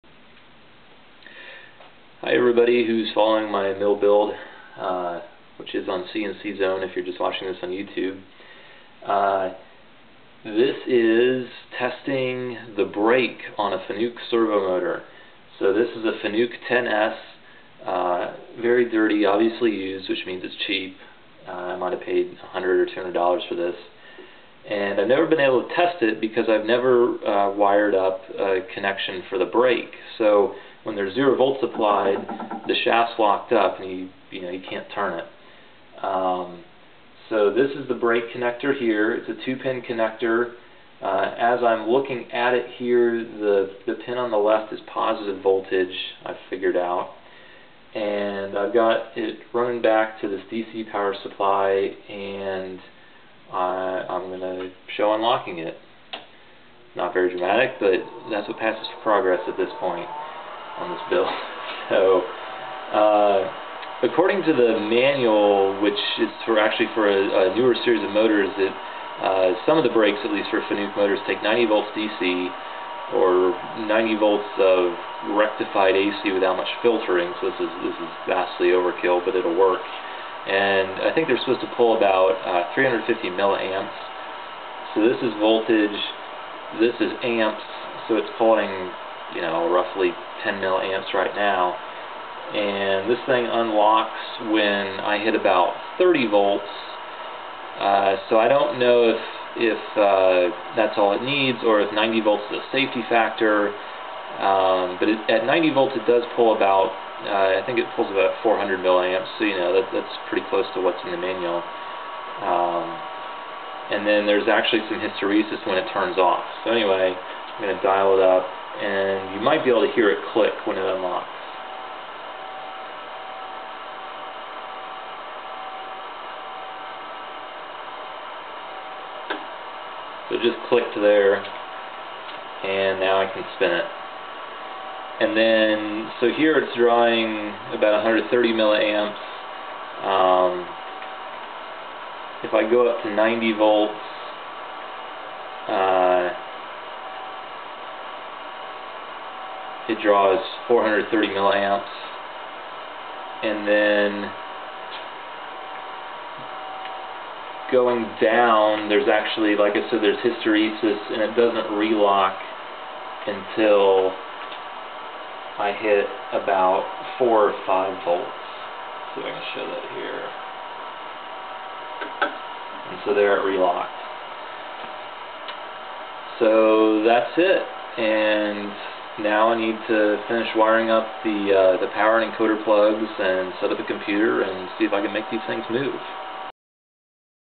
Testing the brake on a Fanuc AC servo
Unlocking the brake for the first time on a Fanuc 10S AC servo motor.  This servo along with a few others will be used on the milling machine I'm designing and building.